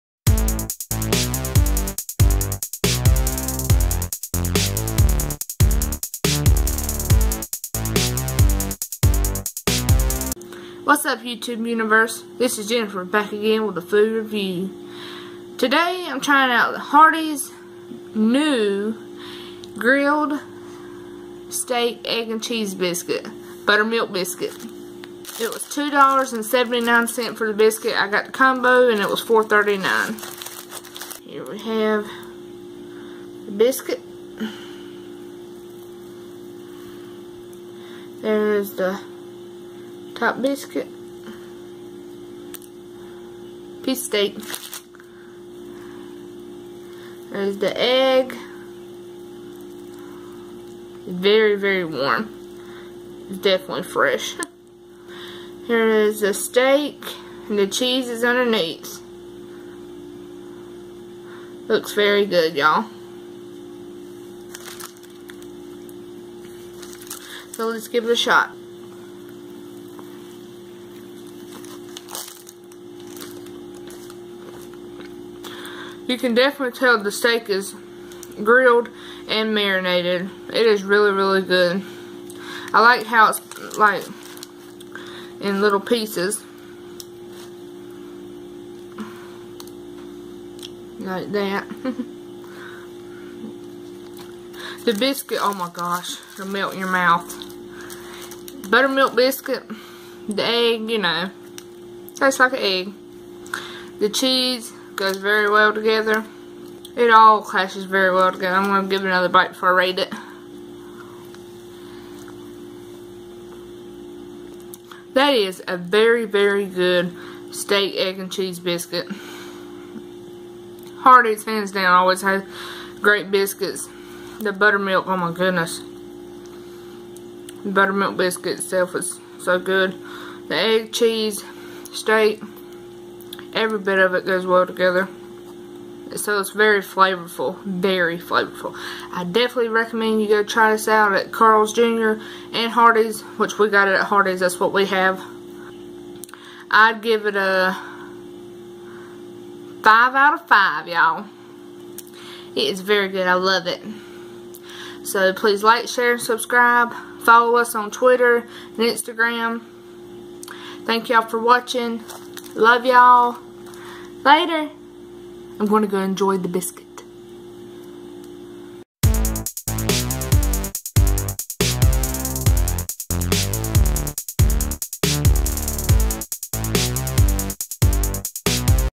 New Hardee's & Carl's Jr STEAK, EGG & CHEESE BISCUIT Review
I give Hardee's & Carl's Jr STEAK, EGG & CHEESE BISCUIT a Review